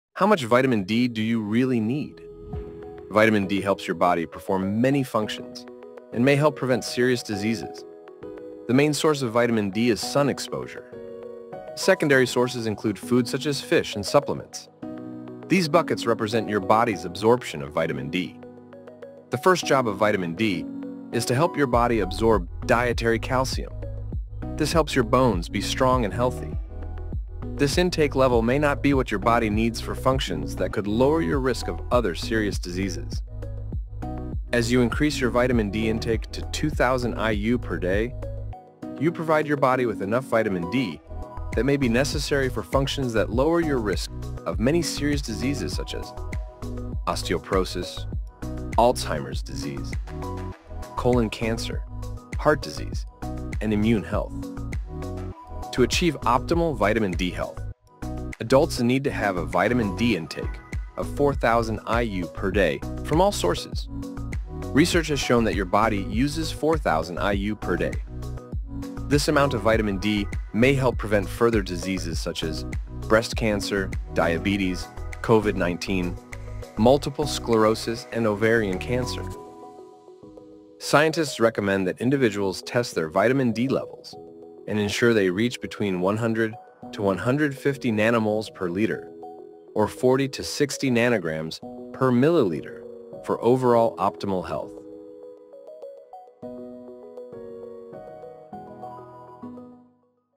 Vitamin D helps your body perform many functions and may help prevent serious diseases
Learn how much vitamin D you need